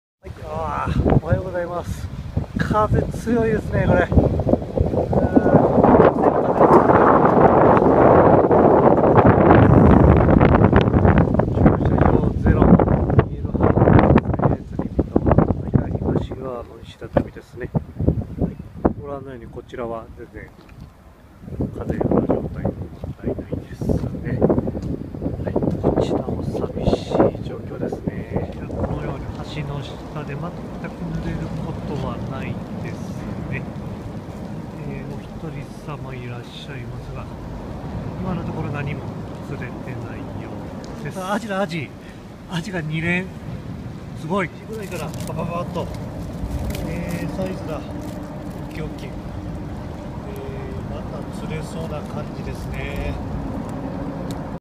fishingmax芦屋 アジ、キタァ～
風裏は寒さもなく快適です。もうすぐすっきりした天気になる予報。
そこから期待ですね。